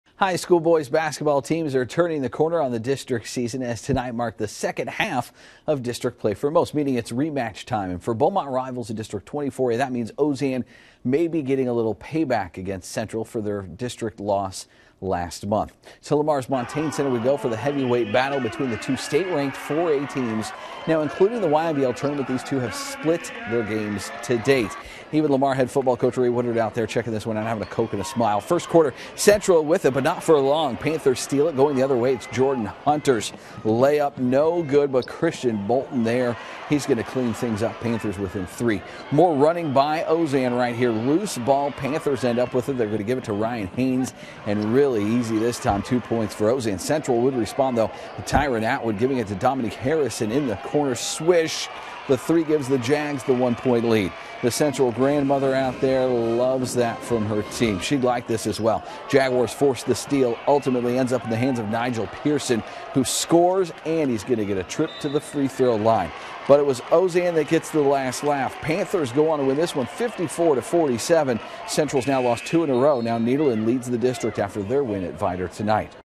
H.S. Boys Hoops - 20-4A Ozen 54 Central 47
#13 Ozen hands #2 Central it's second straight District 20-4A loss. The Panthers have beaten the Jags twice this season including in the YMBL Tournament.